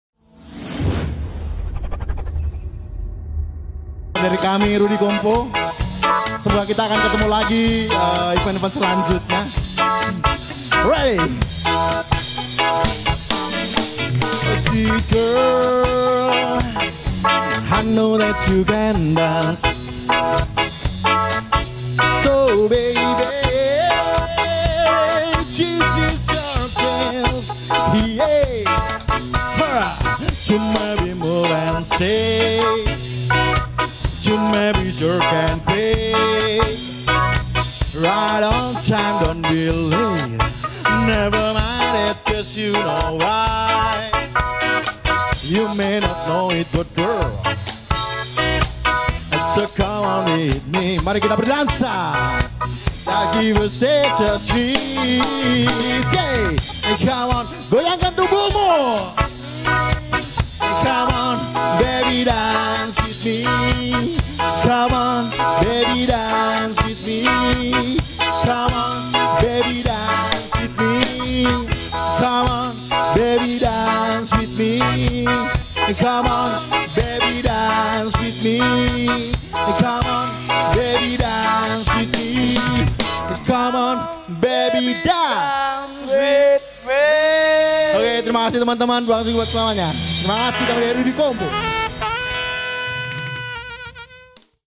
RUDI COMPO BAND - PAGELARAN MUSIK DAN SENI TRADISIONAL MENYAMBUT TAHUN BARU 2019
RUDI COMPO BAND - PAGELARAN MUSIK DAN  SENI TRADISIONAL DALAM RANGKA MENYAMBUT TAHUN BARU 2019

Dalam rangka menyambut tahun baru 2019 dan turut serta memupuk kreatifitas pelajar dan masyarakat umum DINAS KEBUDAYAAN DAN PARIWISATA KOTA SALATIGA mengadakan sebuah pagelaran musik dan seni tradisional. Diselenggarapak pada tanggal 30 - 31 Desember 2019 di lapangan Pancasila Salatiga.